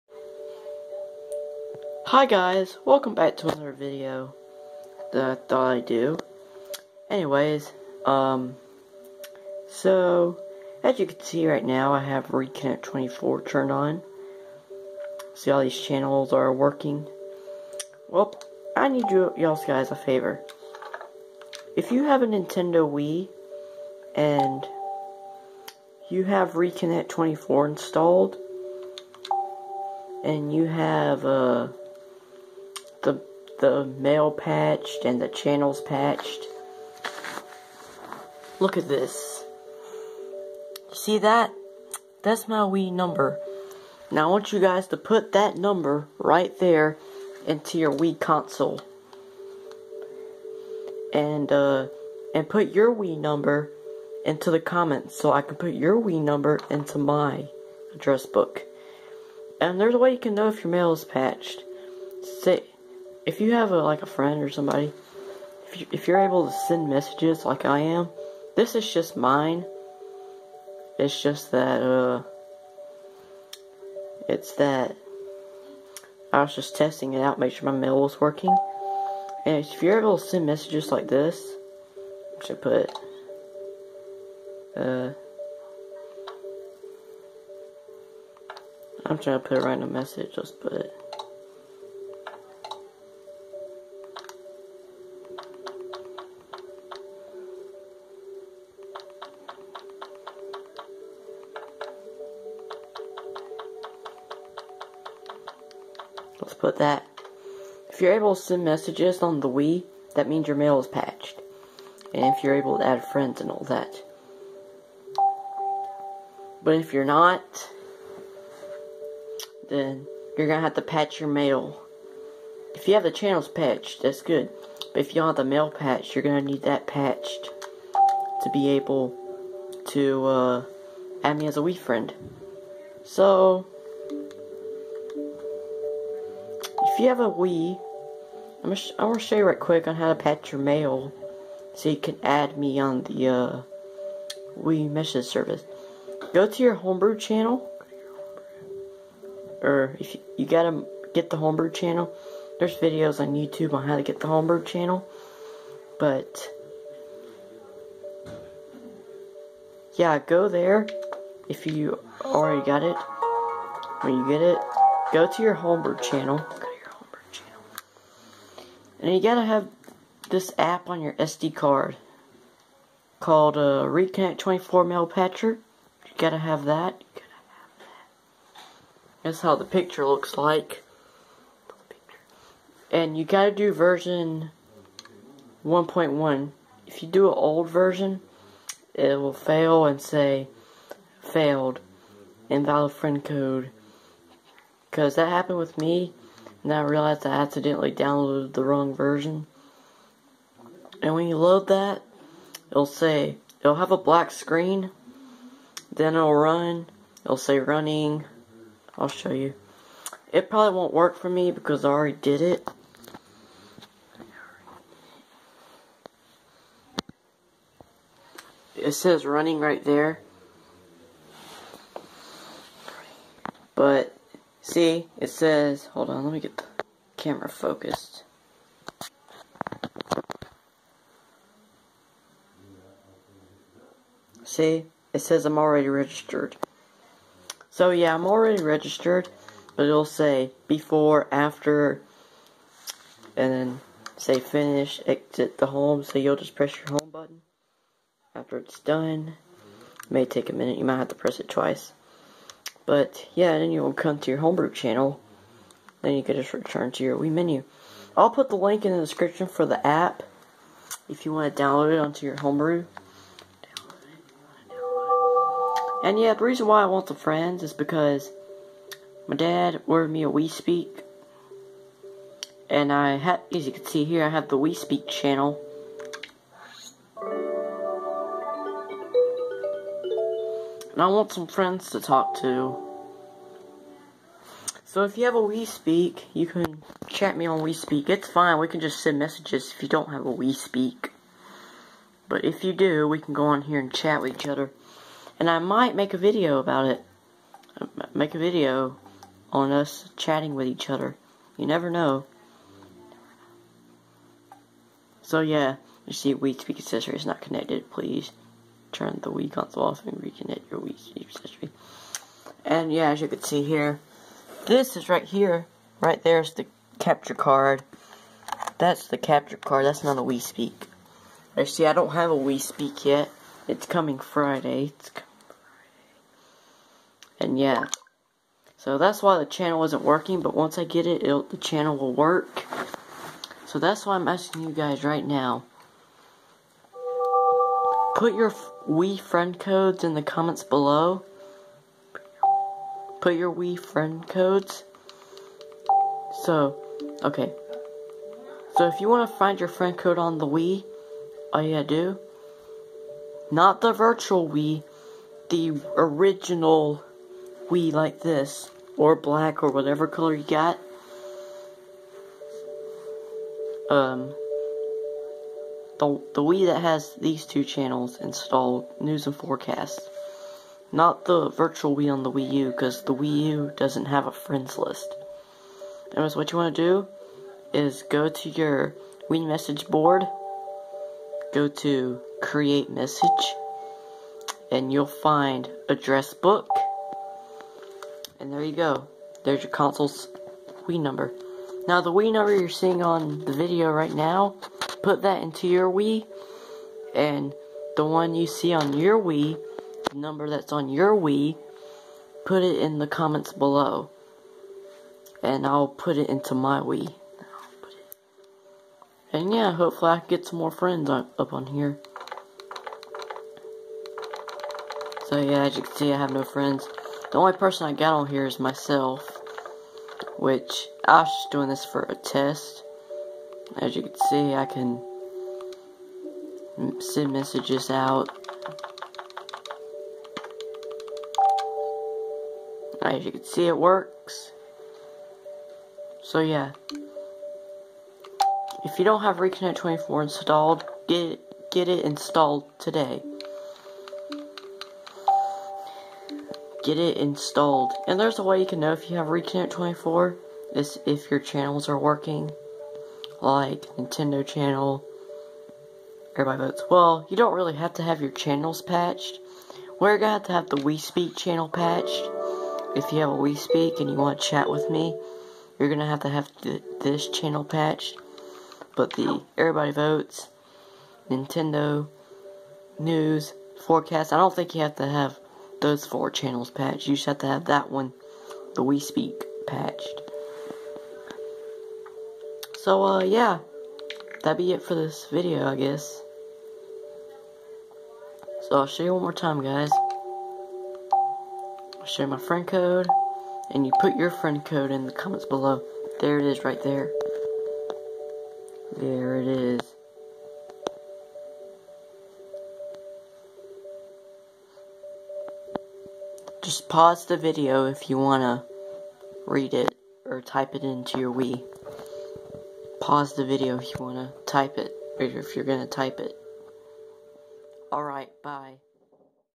Add me as a Wii Friend if you have Riiconnect24
Go to V1.1(Don't click on it) scroll down until you see assets click it then click Mail-Patcher. v1.1.zip then once the zip file is downloaded extract it to the apps folder that is on your SD card. Then put the SD card into your Wii and load the Homebrew Channel then click Riiconnect24 Mail Patcher then it will show a black screen wait 3 or 5 seconds then follow the on screen instructions.